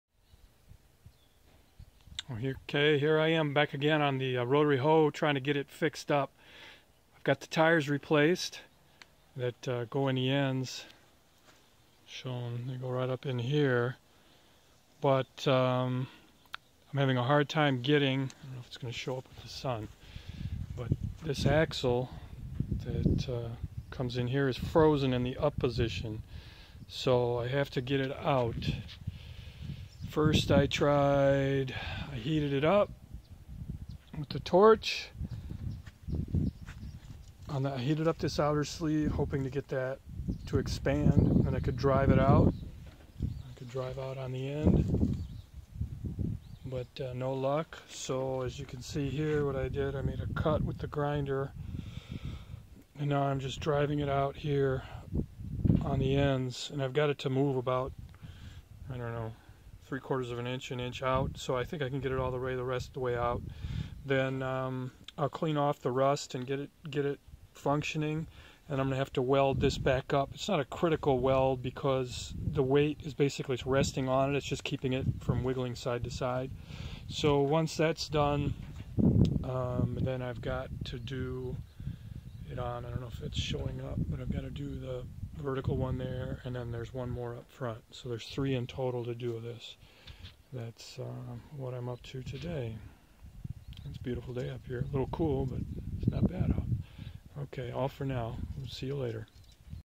Repairing rusted transportation axles on Rotary Hoe
John Deere 614. The moveable transport tires were rusted in the "up" position. I modified the sleeves so the tires could be used, up or down.